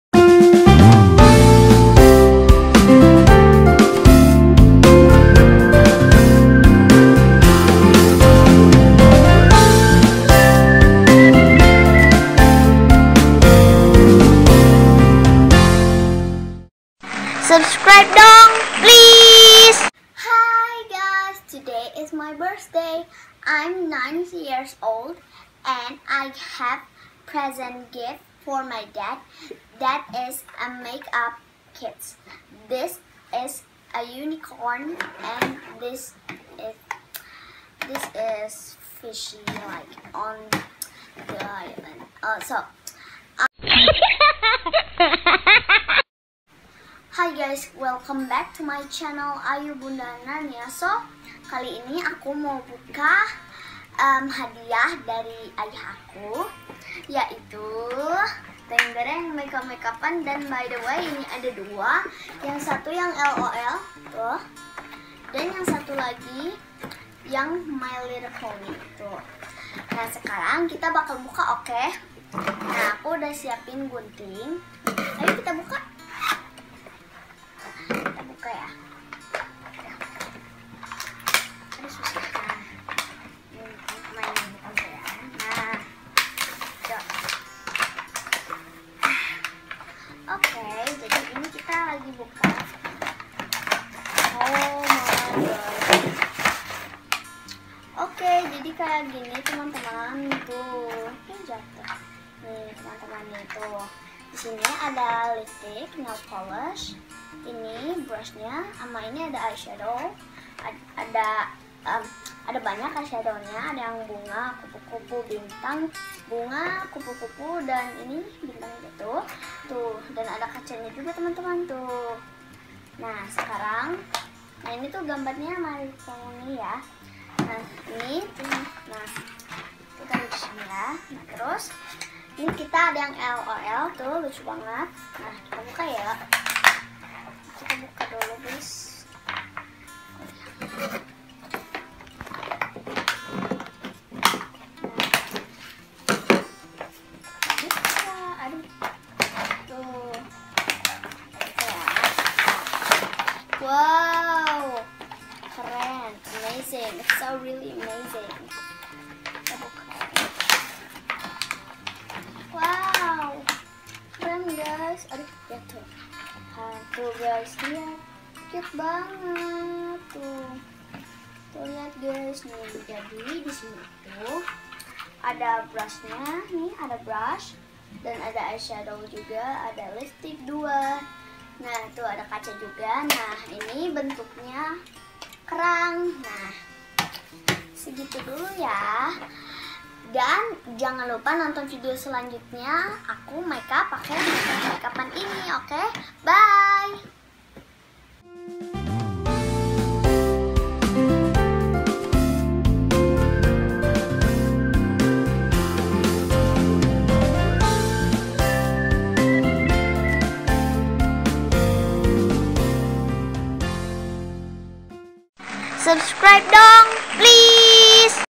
Keseruan kaka buka hadiah makeup🤣🤣
Senangnya dapat makeup dari ayah buat main